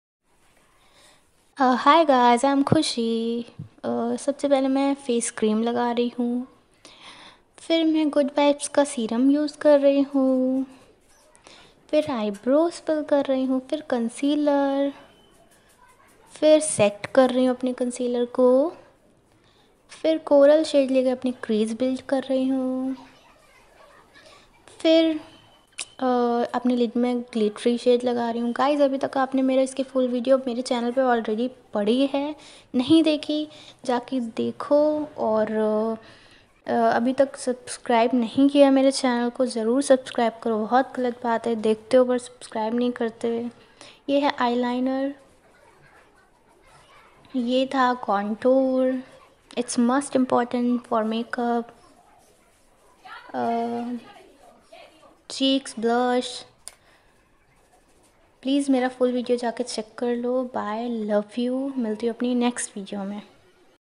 Party Function Makeup Look
Doston Aaj ki video mein Maine aapko Day Time Party function makeup look create karke dikhaya hai ummeed hai aapko ye video pasand aayegi

For Face:-
Good vibes rose hip serum
dermdoc skin tightening face cream
huda beauty primer
me on oil control concealer
swiss beauty 12Ultra Professional Eyeshadow Pallet

For Eyes:-
NY Bae Concealer
Huda Beauty Nude Eyeshadow Pallet
Blue Heaven Waterproof Eyeliner
Ny Bae White Kajal

For Brows:-
Blue Heaven kajal

For Lips
Bella Voste heer Creme Lust Lipstick Mystery Glory